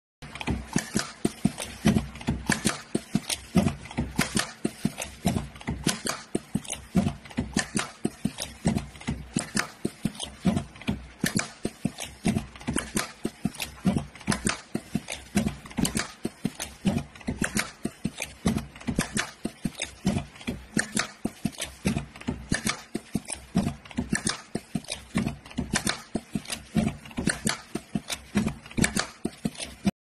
Simplify Product Labeling with Our Auto Tag Feeding Machine | Fast & Efficient!
Transform your product labeling process with our latest model Tag Feeding Machine! Designed for speed and efficiency, this compact and versatile machine makes attaching hang tags to your products quick and effortless.

Perfect for factories producing garments, toys, towels, clothes, and more, our auto tag feeding machine is a must-have for streamlining your operations. Its small size and automated functionality ensure seamless integration into your production line, saving you time and labor costs.

🔧 Key Features:

Fully automated tag feeding system

Compact design, ideal for small to medium-sized factories

Suitable for a wide range of products (garments, toys, towels, etc.)

Upgrade your production process today and experience the convenience of auto tag feeding!